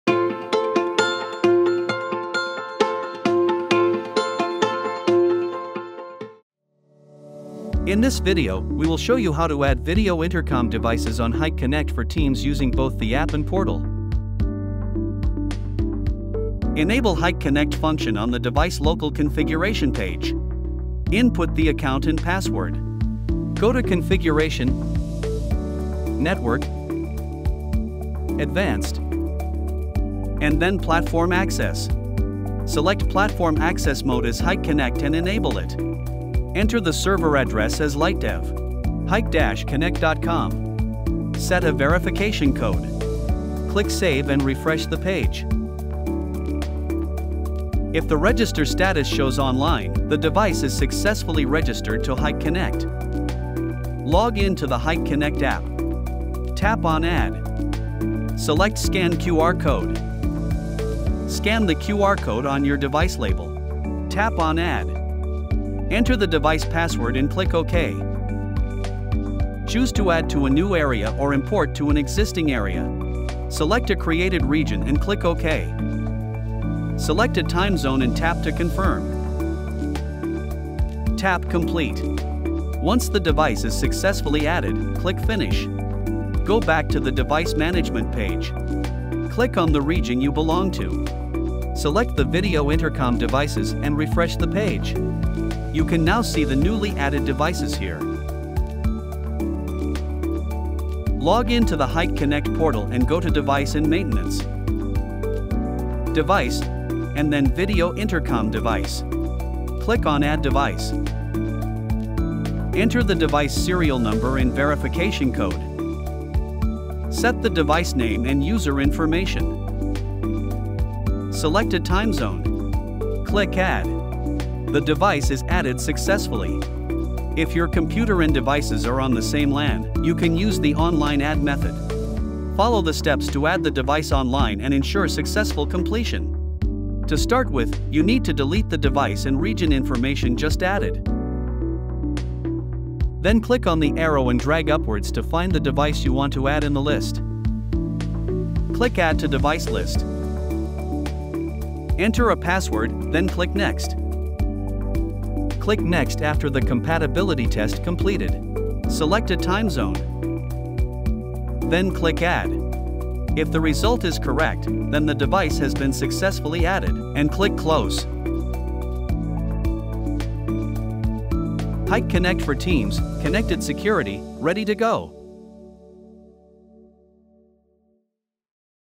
How to Add Video Intercom Devices on Hik Connect For Teams
🎥 Want to add video intercom devices on Hik-Connect for teams? Check out this super helpful tutorial video that breaks down the steps in a friendly and engaging way! Watch it now! HikConnect6 VideoIntercom 🚪🔒

Chapters:
0:00
0:15
Add Video Intercom Devices on Hik-Connect for Teams App
1:39
Add Video Intercom Devices on Hik-Connect for Teams Portal